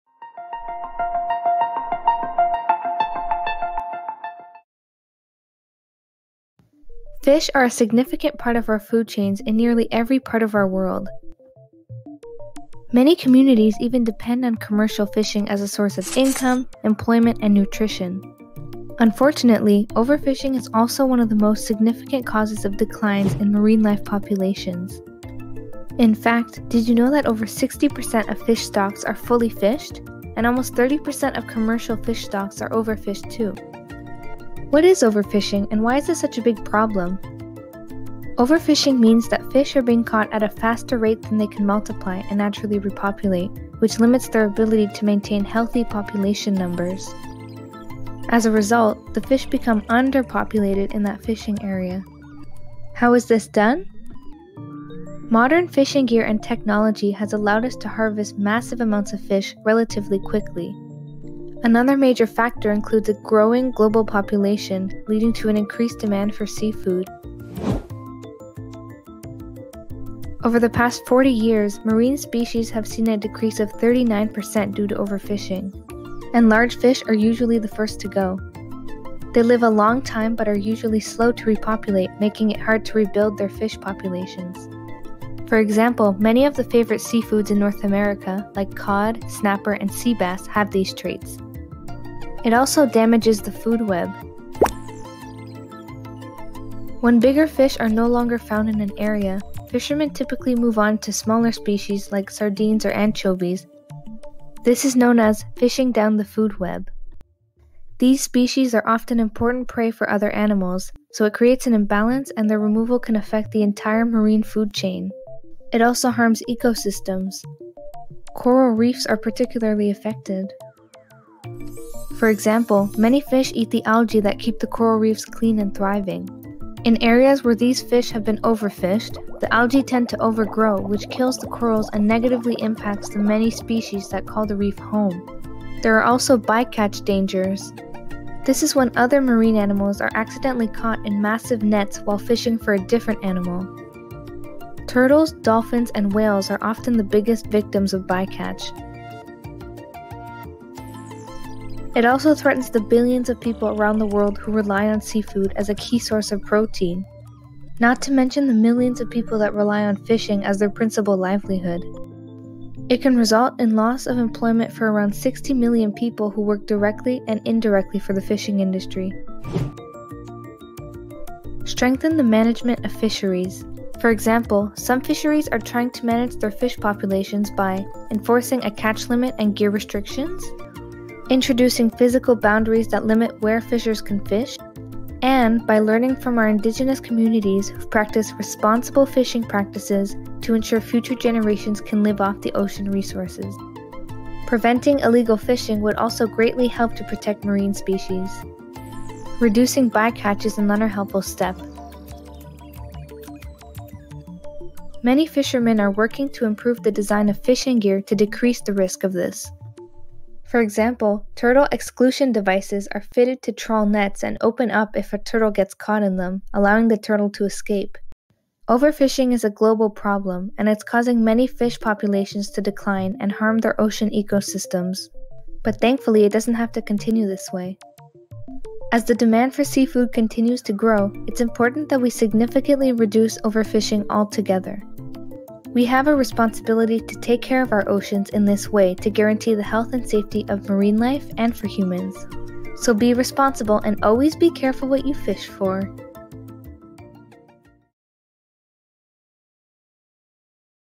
Mission Ocean: Overfishing
In this episode, you’ll discover the concept and threat of overfishing: Why it's a threat, how it’s done, and what modern solutions are helping to combat this ocean danger.